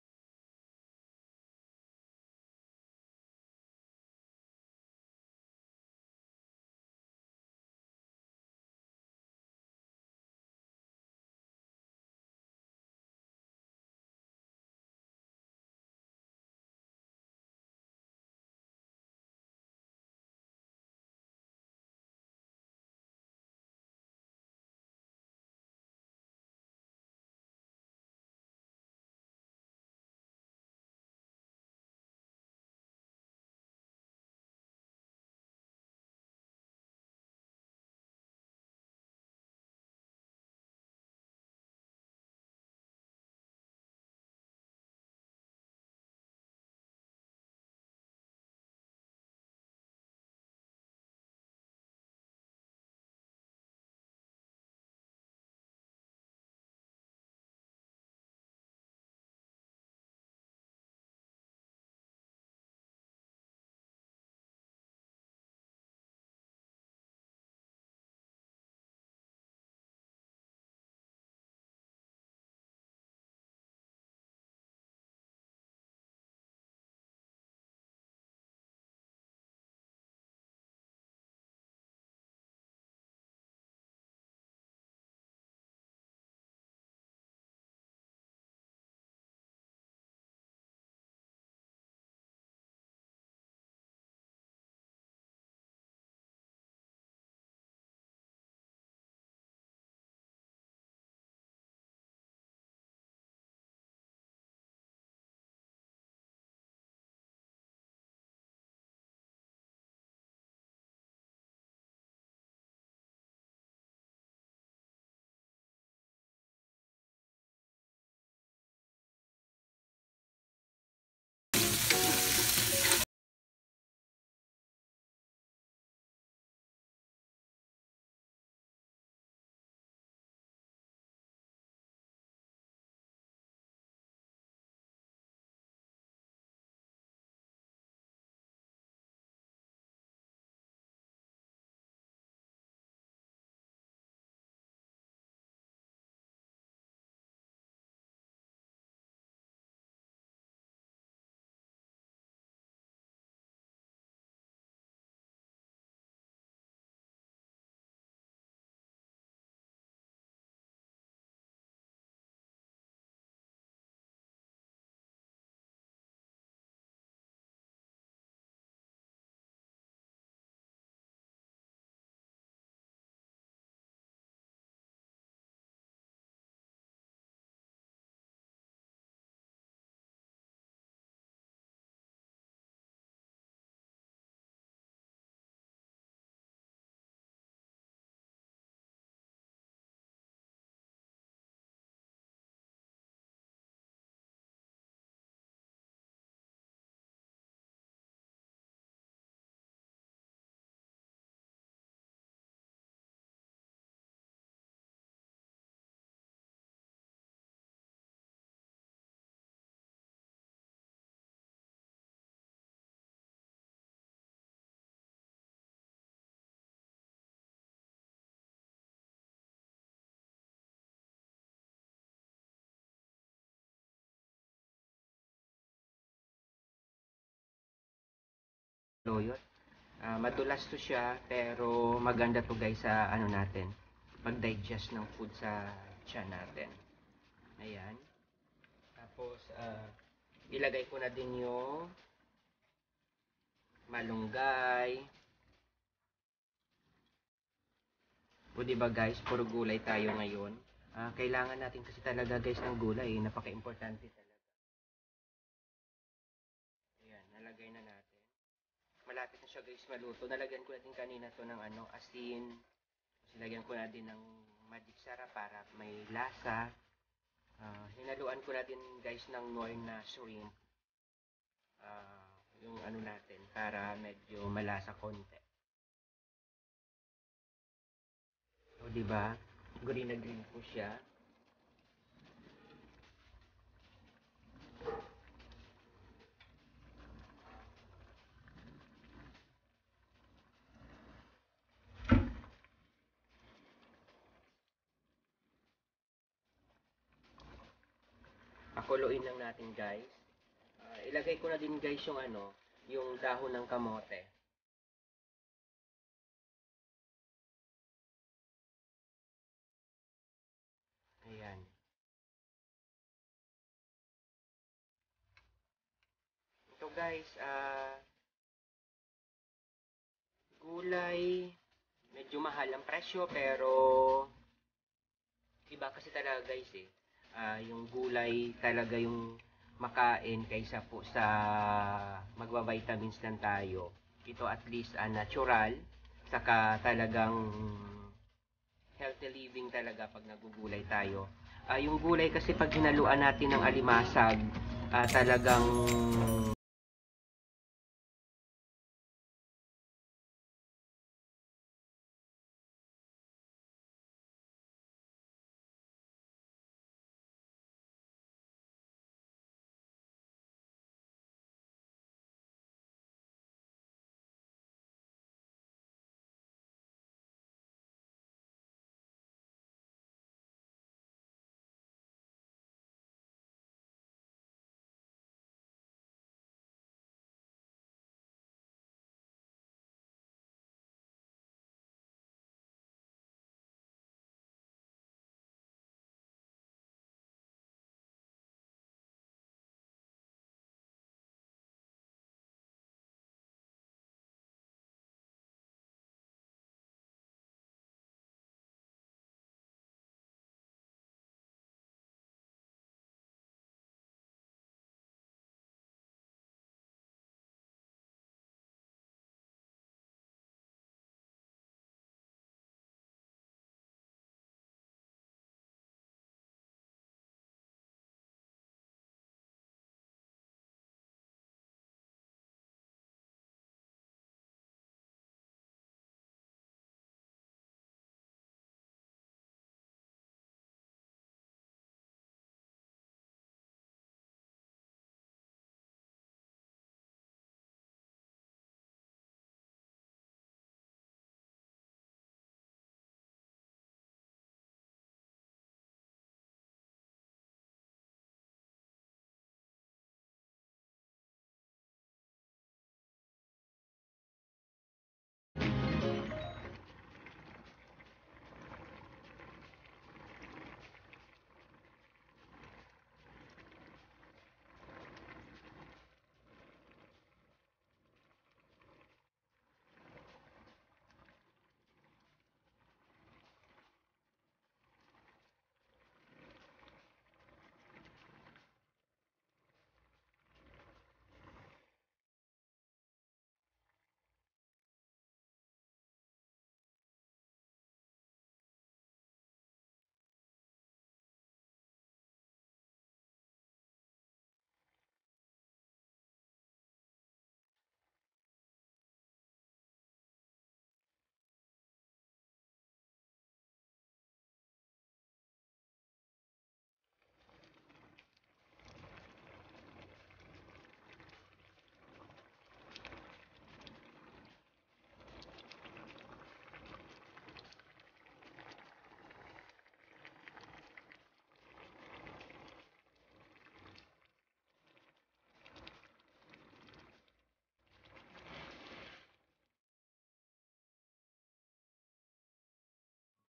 Pagkain na gamot sa lungkot/GULAY PANGPAHABA NG BUHAY
Crabs with vegetables our good to our health. More gulay more life...